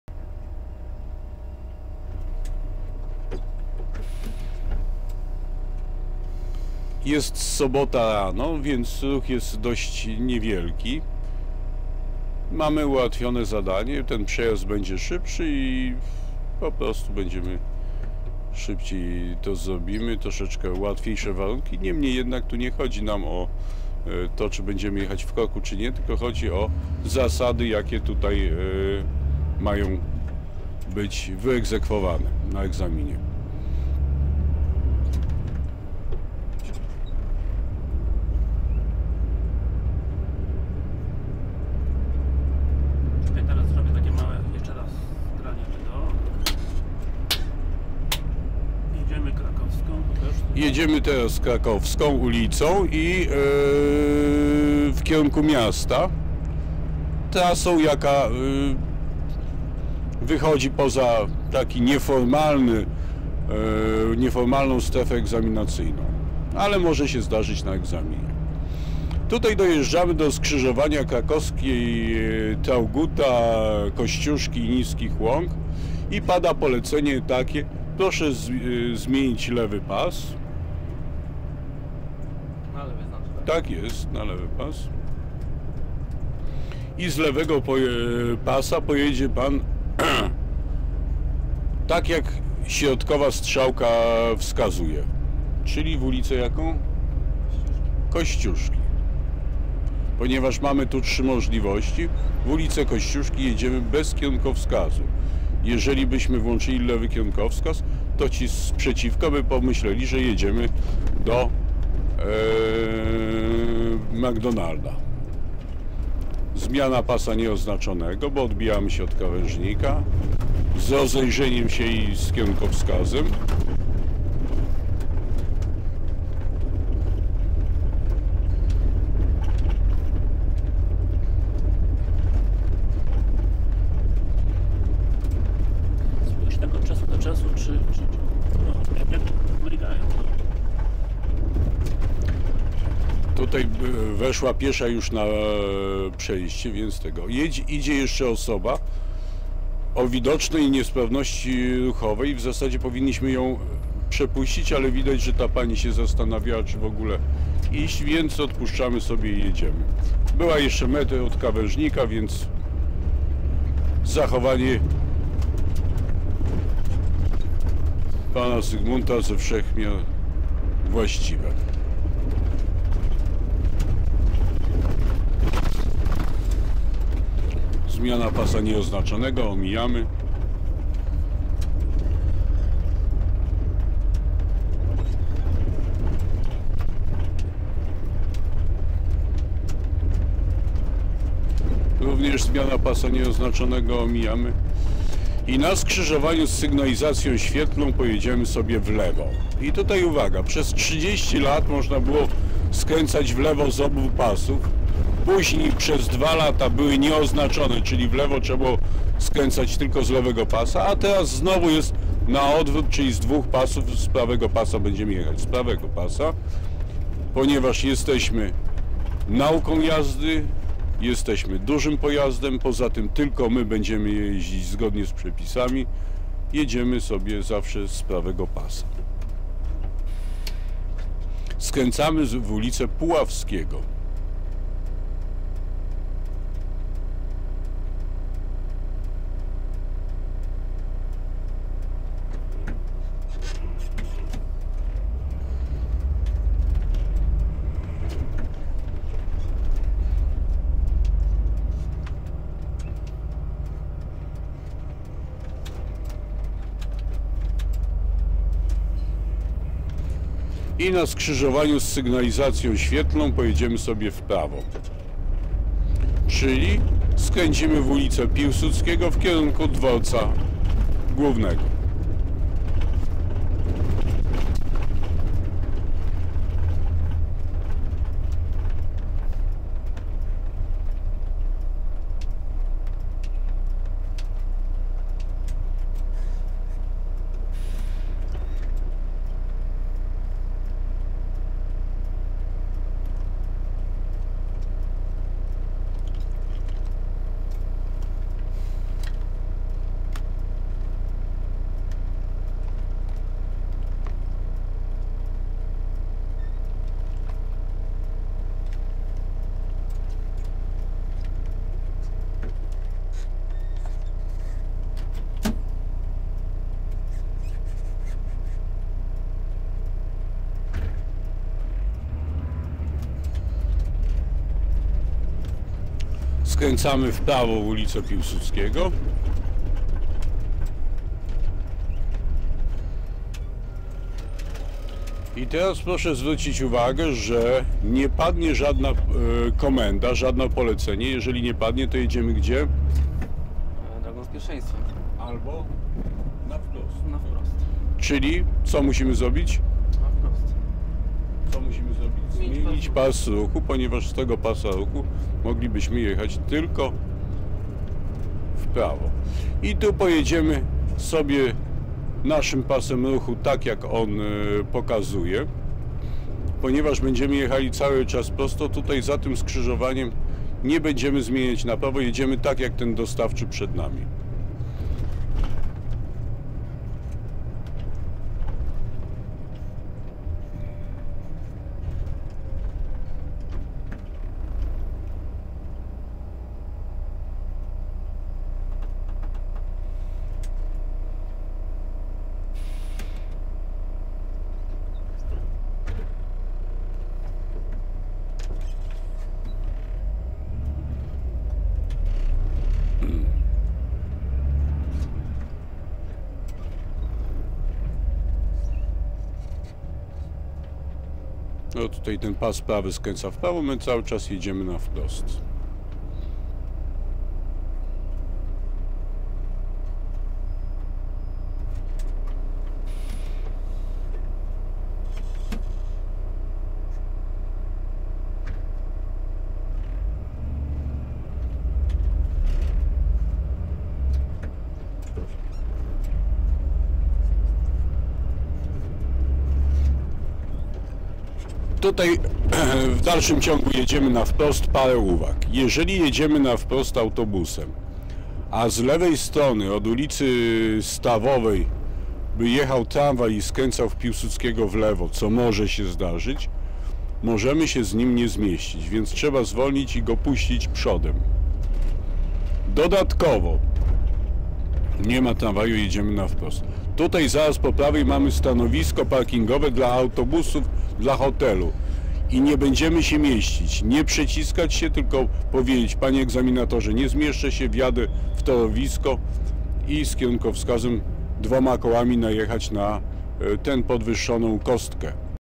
Trasa egzaminacyjna koło Dworca Głównego 1
Szkolony pan Zygmunt naprawdę dawał dzielnie sobie radę i popełnił niewiele błędów, które dyskwalifikowały by go na egzaminie państwowym.

Jazda odbywa się na odcinku Krakowska, Kościuszki oraz Piłsudskiego.

Dariusz Kraśnicki serdecznie zaprasza Cię do komentarzy tego filmu.